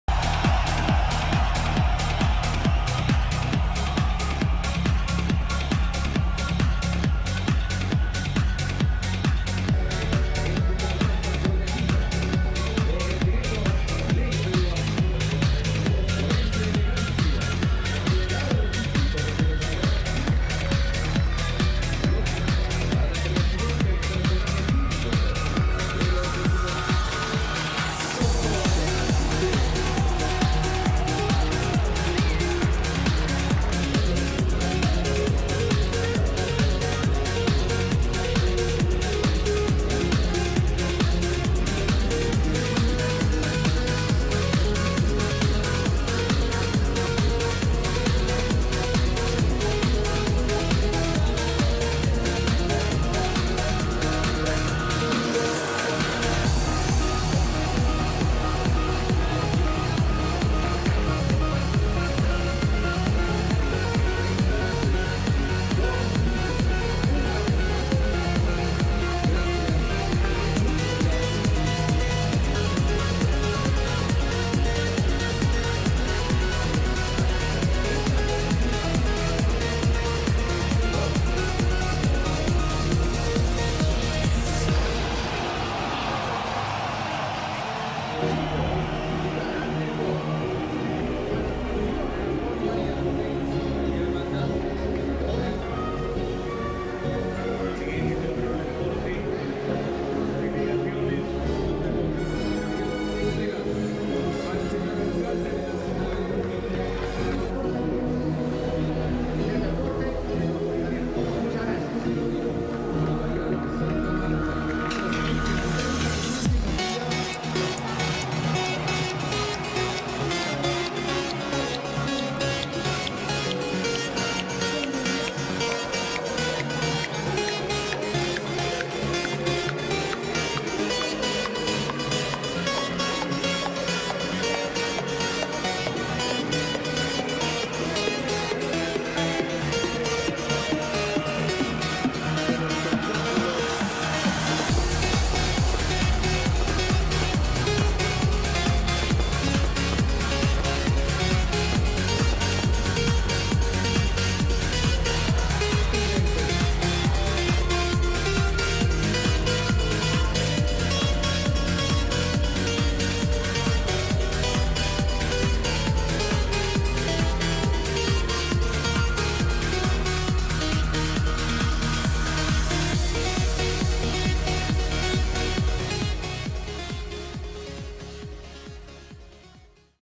Clusura Juegos Bolivarianos Trujillo
Así se dio la clausura de los Juegos Bolivarianos, Trujillo 2013, donde se anuncio la sede de los próximos juegos, Santa Marta, Colombia 2013.